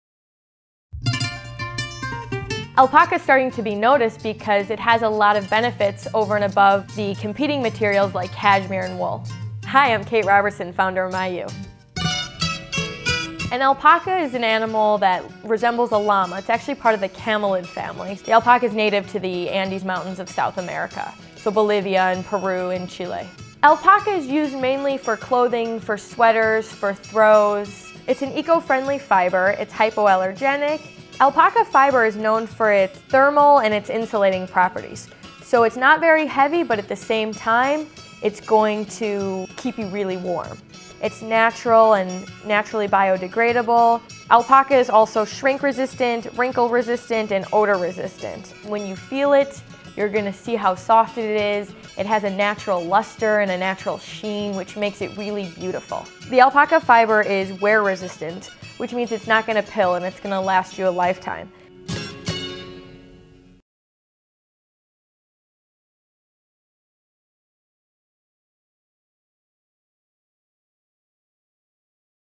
What is Alpaca?
What is Alpaca? Alpaca is starting to be noticed because it has a lot of benefits over some of the competing materials such as cashmere and wool. Hi Im Kate Robertson, founder of Mayu. The alpaca is native to the Andes Mountains of South America, to Bolivia, Peru and Chile. Alpaca is used mainly for clothing, for sweaters, for throws. Its an eco-friendly fiber. Its hypoallergenic. Alpaca fiber is known for its thermal and insulating properties. So, its not very heavy but at the same time its going to keep you very warm. Alpaca is also shrink resistant, wrinkle resistant and odor resistant. When you feel it youre going to see how soft it is. It has a natural luster and a natural sheen, which makes it really beautiful. The Alpaca fiber is wear-resistant, which means its not going to pill and its going to last you a lifetime.